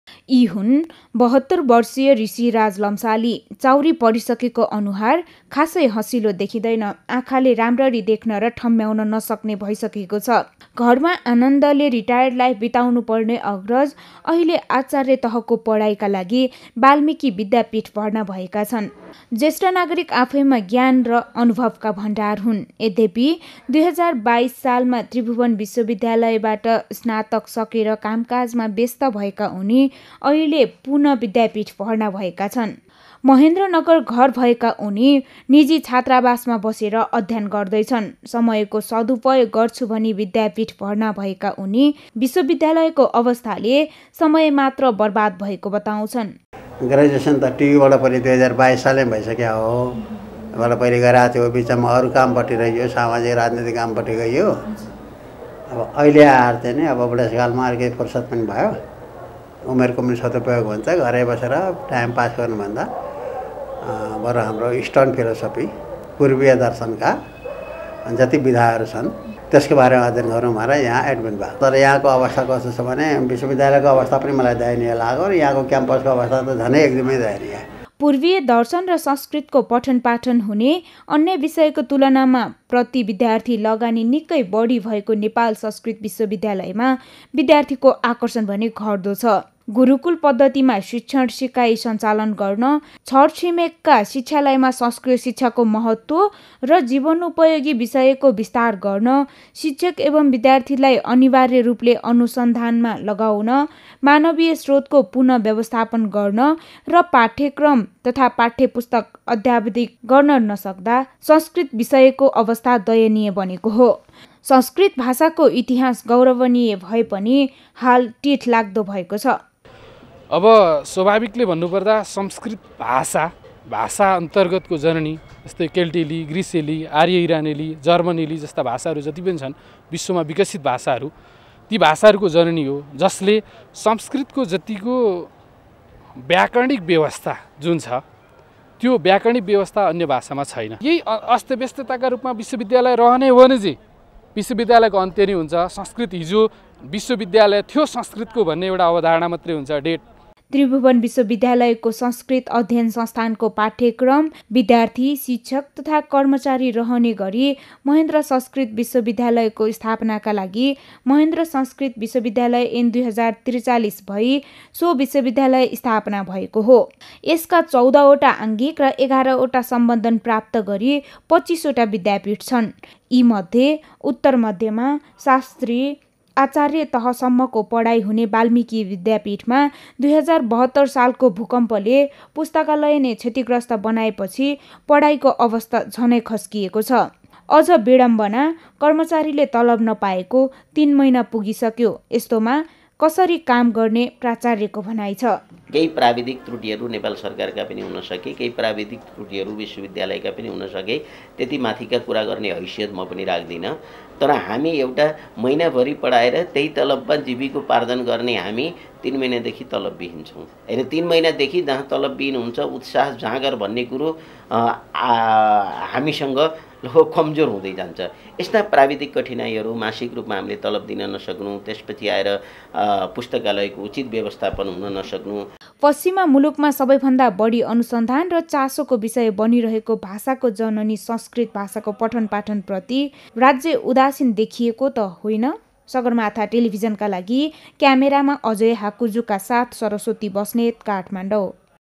संस्कृतमा विद्यार्थीको आकर्षण घट्दा :
अन्य विश्वविद्यालयका तुलनामा प्रतिविद्यार्थी लगानी निकै बढी भएको नेपाल संस्कृत विश्वविद्यालयमा विद्यार्थीको आकर्षण भने घट्दो छ । समयानुकुल संस्कृतको पाठ्यक्रम परिमार्जन नहुनु, त्यसमाथि विश्वविद्यालयका पदाधिकारी, शिक्षक र कर्मचारीको राजनीतिक खिचातानीका कारण विद्यार्थी भने निराश छन्